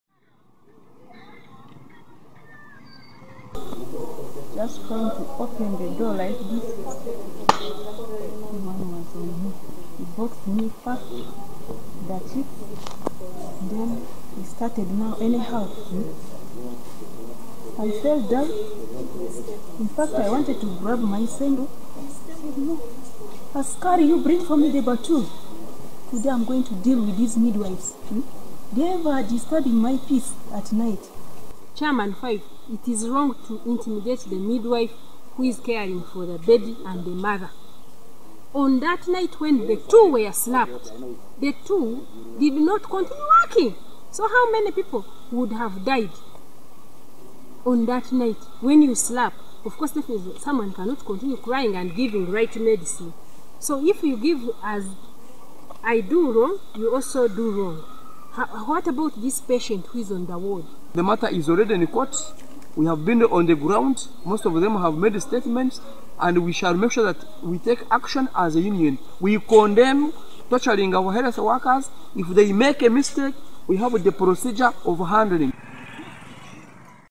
Adjumani medical superintendent in trouble over assault
Uganda Nurses and Midwives Union have appealed to the police to speed up investigations into the administrator of Adjumani Hospital Michael Ojja, who is said to have assaulted several nurses, with one of them sustaining a miscarriage. It is unclear why this nurse was assaulted, however, she is now a patient in the same hospital, recovering from her injuries. The matter has now been referred to the police and the Uganda Nurses and Midwives Union.